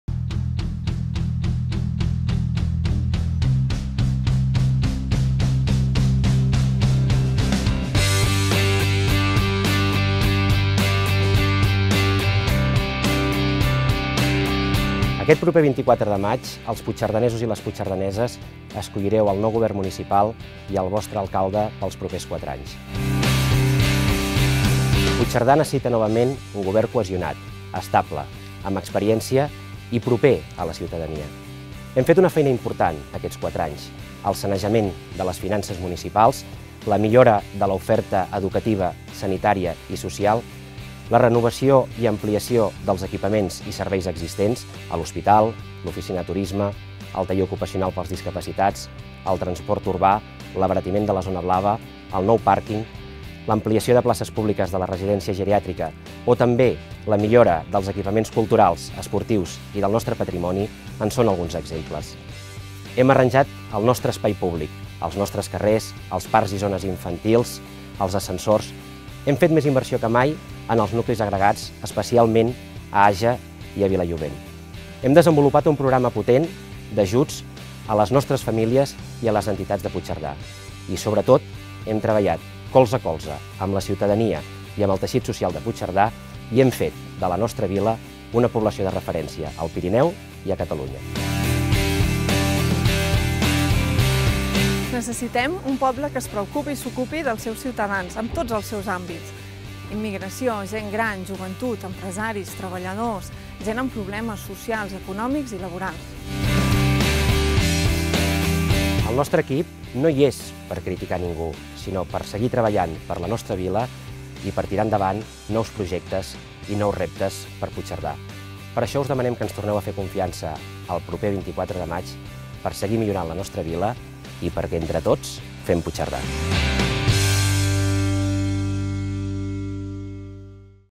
Spot electoral CiU Puigcerdà - Eleccions Municipals 2015
Spot electoral deConvergència i Unió per a les eleccions municipals de Puigcerdà de 2015

Spot electoral de Convergència i Unió para las elecciones municipales de Puigcerdà de 2015

Electoral spot of Convergència i Unió for municipal elections in Puigcerdà in 2015